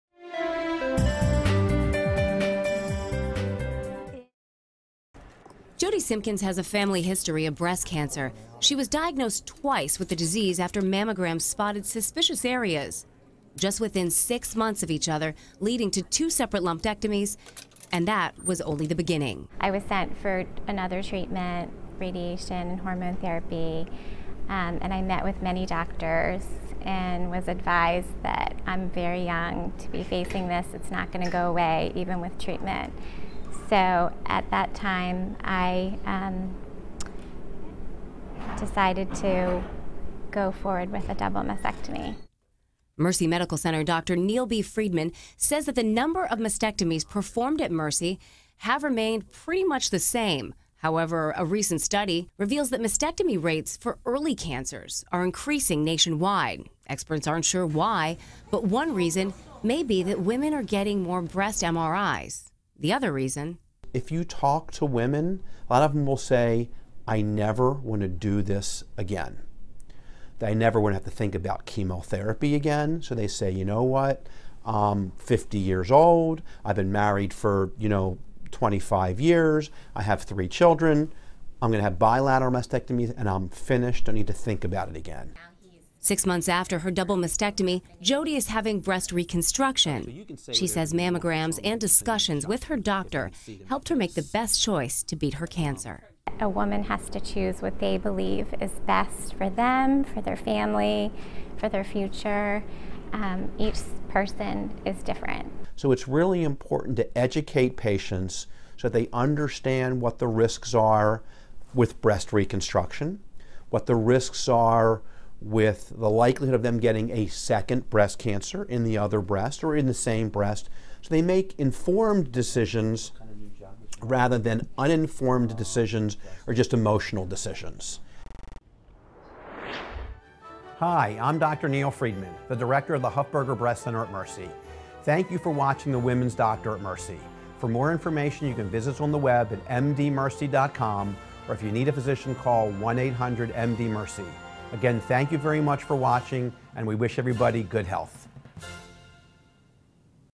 Increase in Mastectomies in Early Cancer Detection - Dr. Neil B. Friedman - Mercy
Mercy breast cancer surgeon Dr. Neil B. Friedman discusses research indicating an increase in mastectomies in early cancer detection.